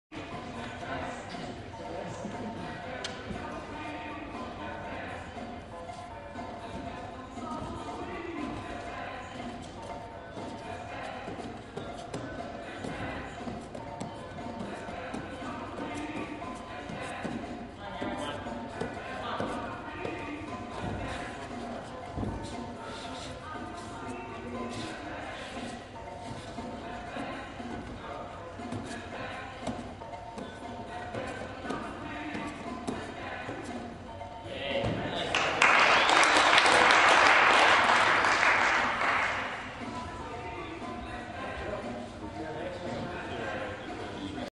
Walls PH
Walls Pommel horse routine at the 2006 Canadian Men's World team Trials (Trials-3 August 31)